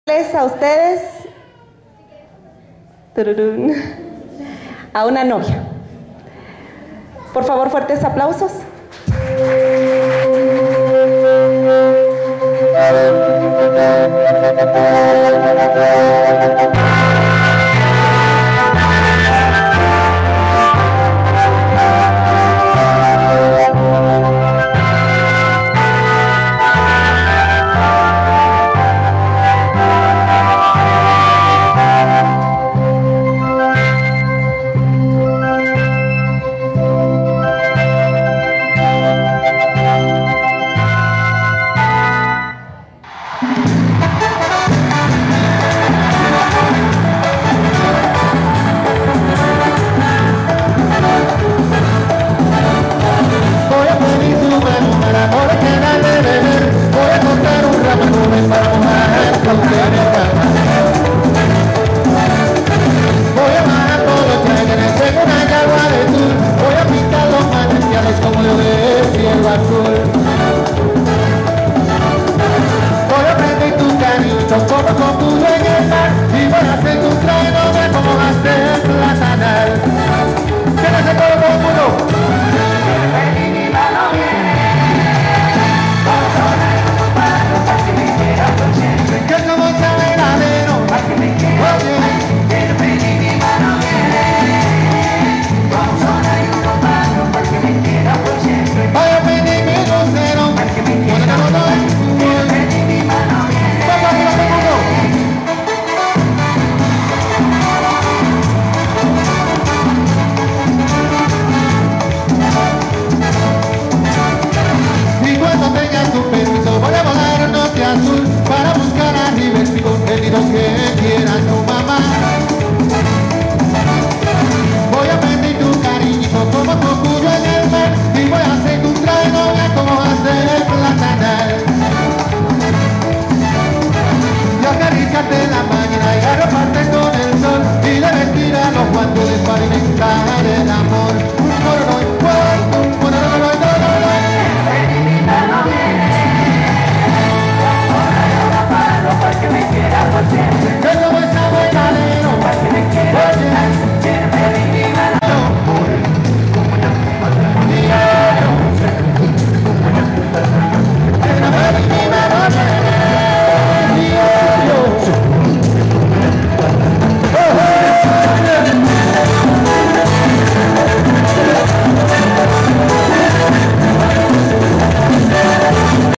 Desfile de novia en la pasarela
Patzún, Desfile de novia en la pasarela en el desfile de moda de trajes típicos de Almacén Betzy, Colección 2014 sobre pasarela